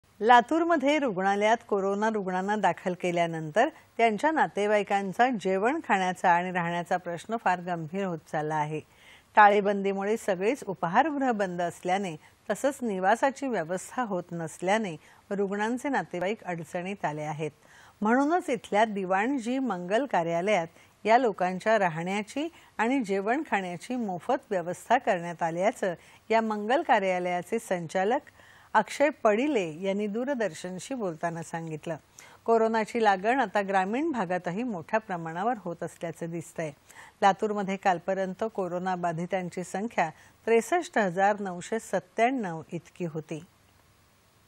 लातूरमध्ये दिवाणजी मंगल कार्यालयात या लोकांच्या राहण्याची आणि जेवणाखाण्याची मोफत व्यवस्था
लातूरमध्ये रुग्णालयात कोरोना रुग्णांना दाखल केल्यानंतर त्यांच्या नातेवाईकांचा जेवणाखाण्याचा आणि राहण्याचा प्रश्न फार गंभीर होत चालला आहे.  टाळेबंदीमुळे सगळीच उपाहारगृह बंद असल्यानं तसंच निवासाची व्यवस्था होत नसल्यानं रुग्णांचे नातेवाईक अडचणीत आले आहेत.  म्हणूनच इथल्या दिवाणजी मंगल कार्यालयात या लोकांच्या राहण्याची आणि जेवणाखाण्याची मोफत व्यवस्था करण्यात आल्याचं या मंगल कार्यालयाचे संचालक अक्षय पडिले यांनी दूरदर्शनशी बोलताना सांगितलं. कोरोनाची लागण आता ग्रामीण भागातही मोठया प्रमाणावर होत असल्याचं दिसतंय, लातूरमध्ये कालपर्यंत कोरोनाबाधितांची संख्या ६३ हजार ९९७ इतकी होती.